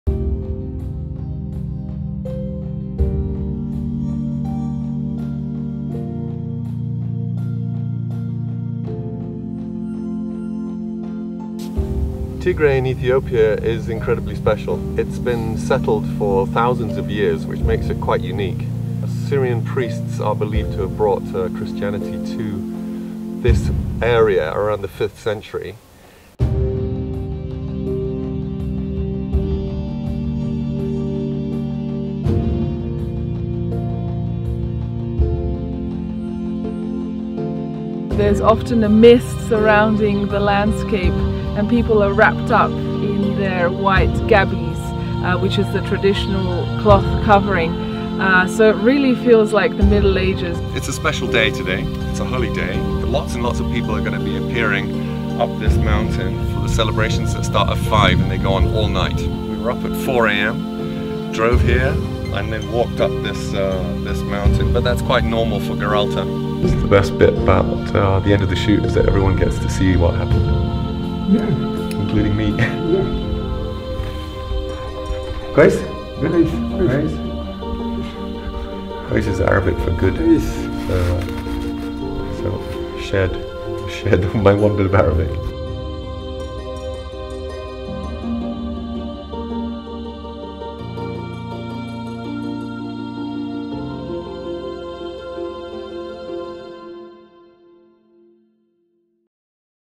Lights, Camera, Africa - Tigray
On our epic Ethiopian adventure, the second big stop or destination was the Tigray highlands. Here we met an ancient culture and visited rock hewn churches that were created around the 11-12th centuries.